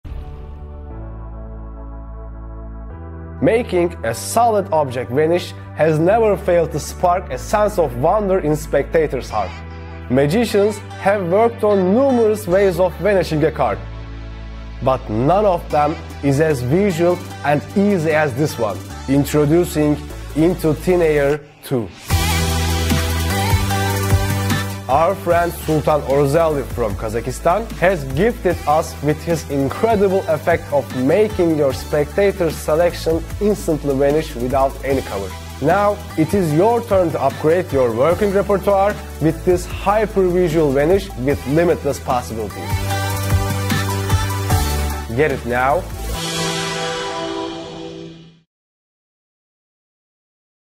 Into Thin Air 2.0 by Sultan Orazaly
Een voorwerp laten verdwijnen is en blijft voor de mensen iets heel magisch.
Er zijn dan ook veel manieren om bijvoorbeeld een kaart te laten verdwijnen, maar geen versie is zó visueel als deze!

Dit is Into Thin Air 2.0 van Sultan Orazaly uit Kazakhstan.
Hoewel het een waanzinnig op zich zelf staand effect is, kun je dit prima combineren in een routine en je routine nóg sterker maken: Kaart naar locatie, een Sandwich, Card on Ceiling, etc.

Ook jij kunt nu je kaarttrucs upgraden door Into Thin Air 2.0 toe te voegen aan je repertoire

Wat krijg je:
Een professioneel gemaakte (Bicycle) gimmick en een instructie DVD.
Zorg zelf voor een regulier spel kaarten, maar die heb je vast al wel.

Zoekwoorden:
Goochelen
goocheltrucs
goochelwinkel
goocheltrucs leren
goochelen met uitleg
goochelen doe je zo